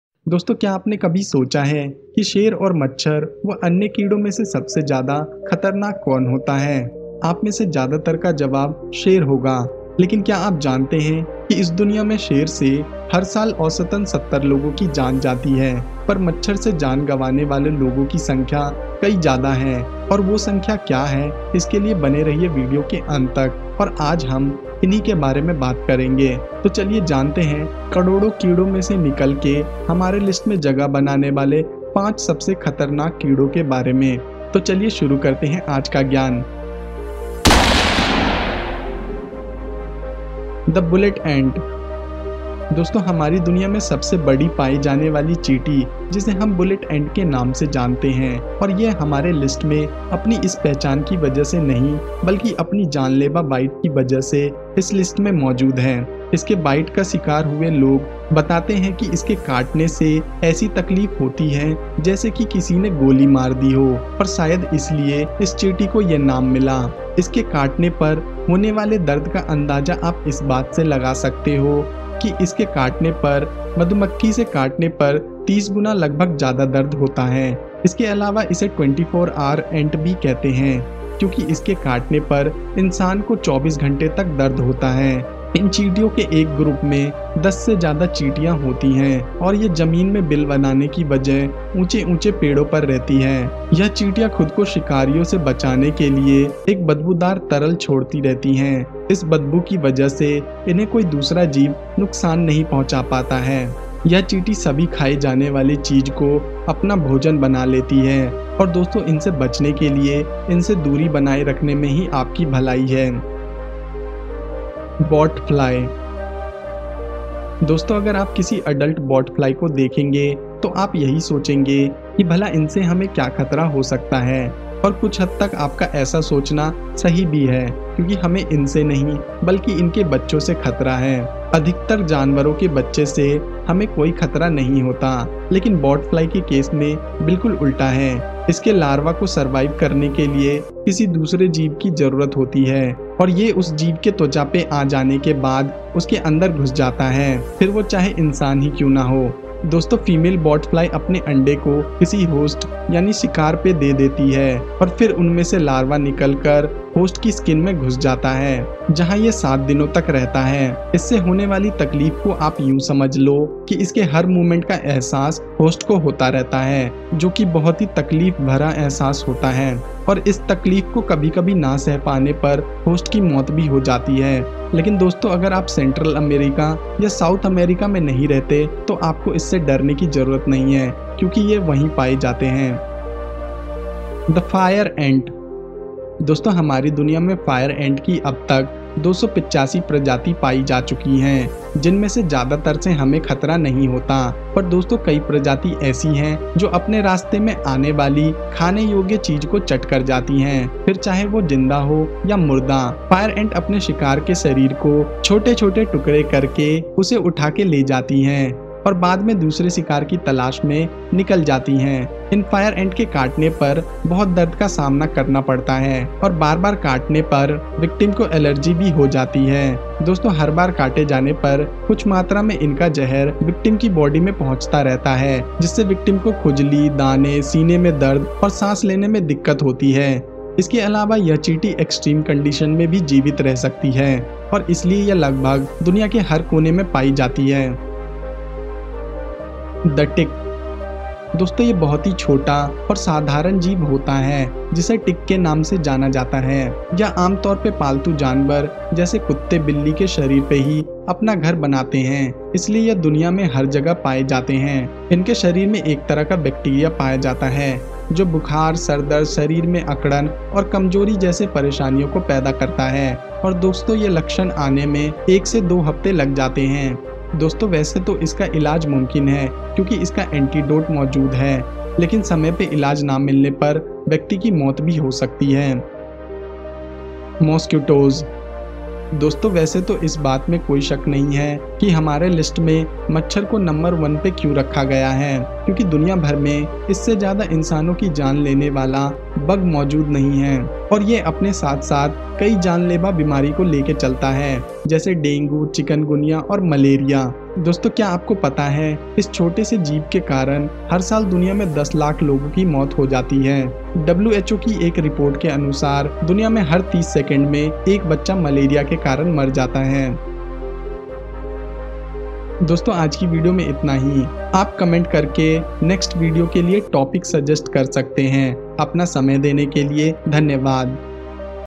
देखिये कैसे ये दुनिया के 5 सबसे खतरनाक कीड़े आपकी जान ले सकते है | 5 MOST Dangerous Bugs
Be Happy Keep Smile :) me